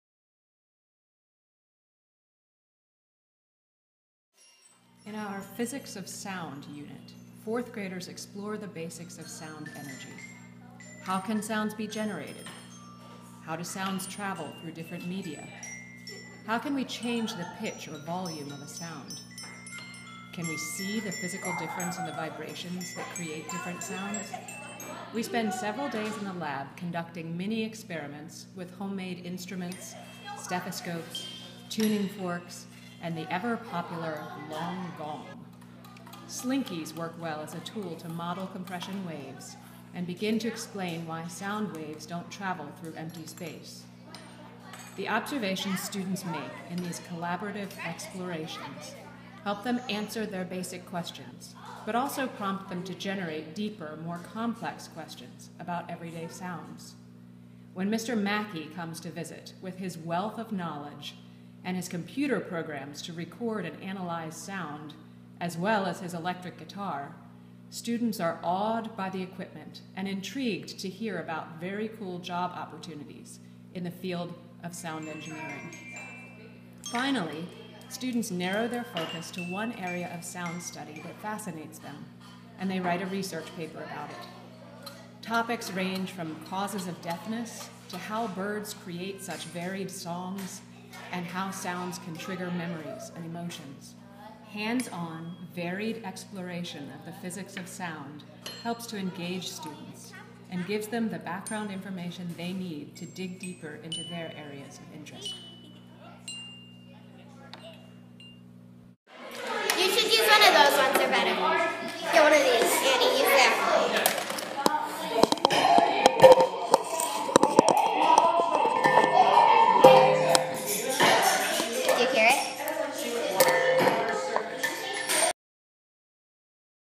Physics of Sound 2017
Fourth graders explore the physics of sound in this fun lab...